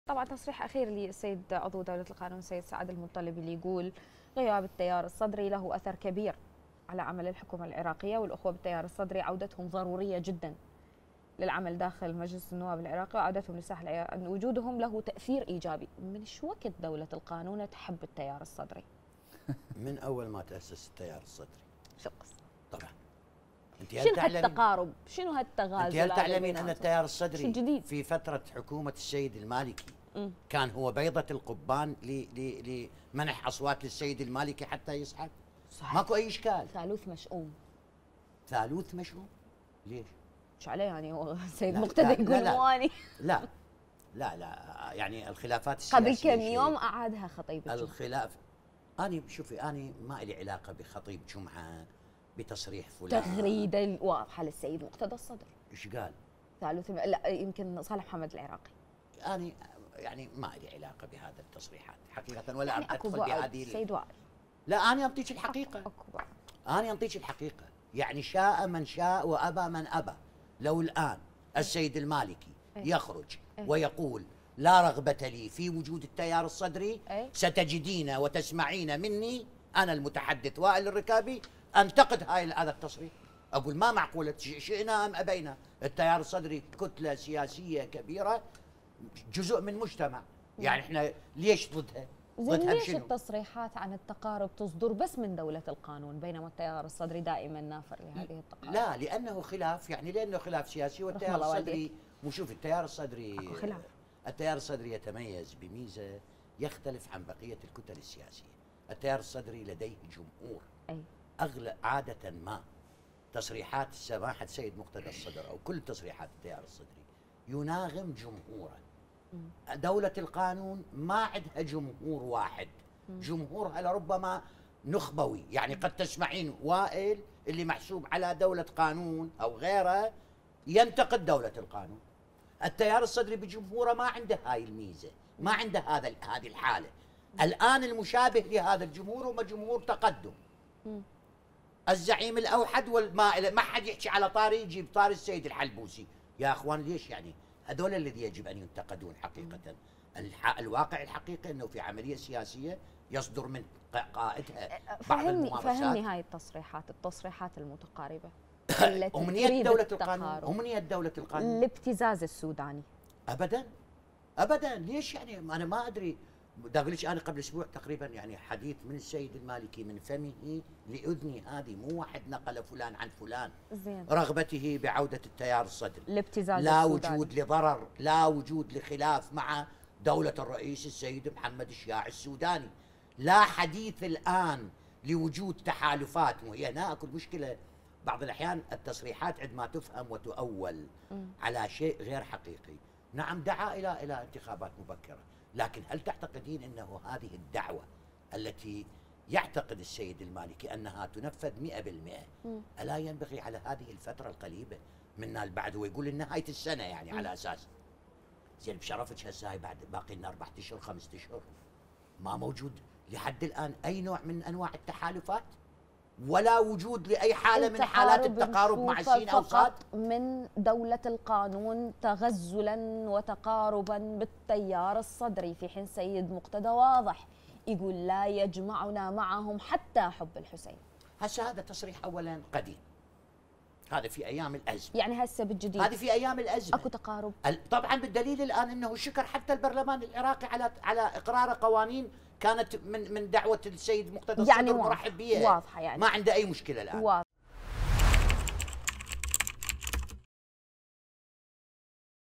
المحلل السياسي وائل الركابي يكشف عن رغبة المالكي بعودة التيار الصدري للعملية السياسية
"دولة القانون جمهورها نخبوي والتيار يناغم جمهوره".. المحلل السياسي وائل الركابي يكشف عن رغبة المالكي بعودة التيار الصدري للعملية السياسية